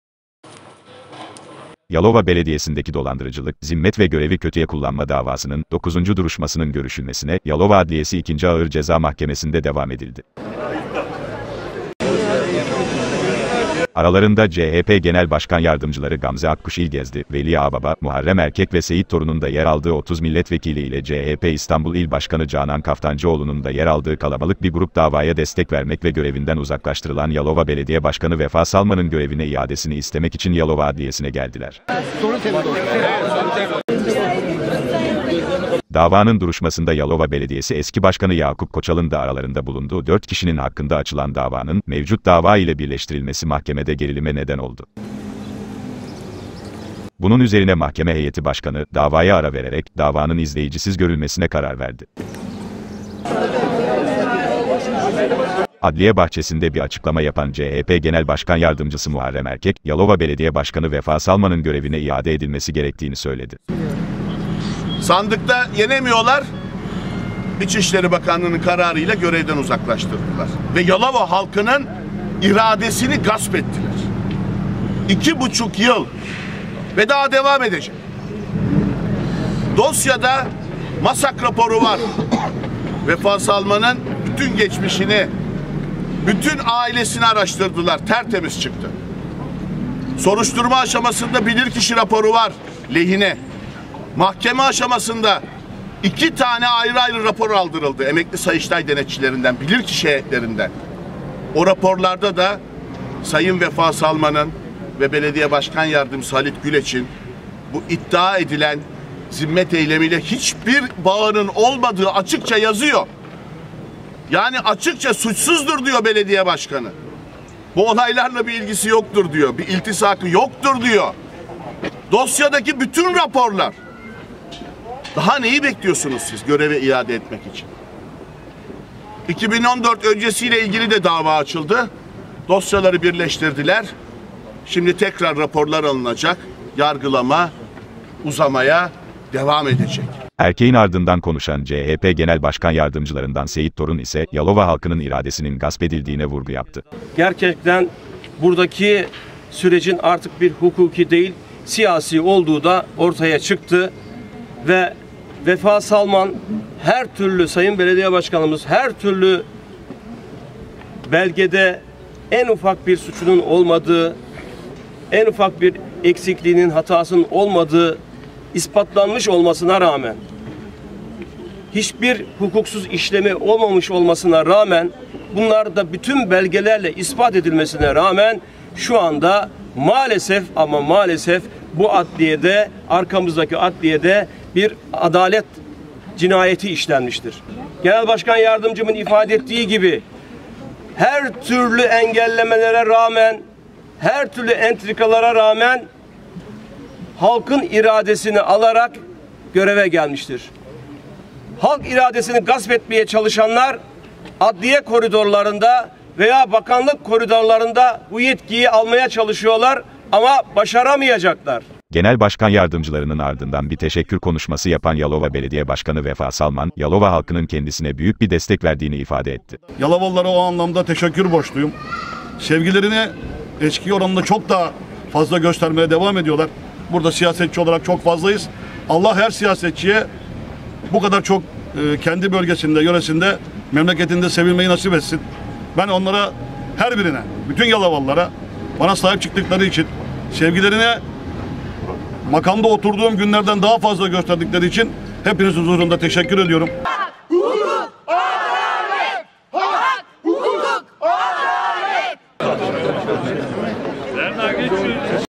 DAVALARIN BİRLEŞTİRİLMESİ GERGİNLİĞE NEDEN OLDU
Yalova Belediyesi’ndeki dolandırıcılık, zimmet ve görevi kötüye kullanma davasının 9. Duruşmasında Yalova Belediyesi eski Başkanı Yakup Koçal’ın da aralarında bulunduğu 4 kişinin hakkında açılan davanın da mevcut dava ile birleştirilmesi mahkemede gerilime neden oldu. Davaya katılmak üzere Yalova’ya gelen CHP’li vekiller hakime tepki gösterince hakim salonu boşalttı.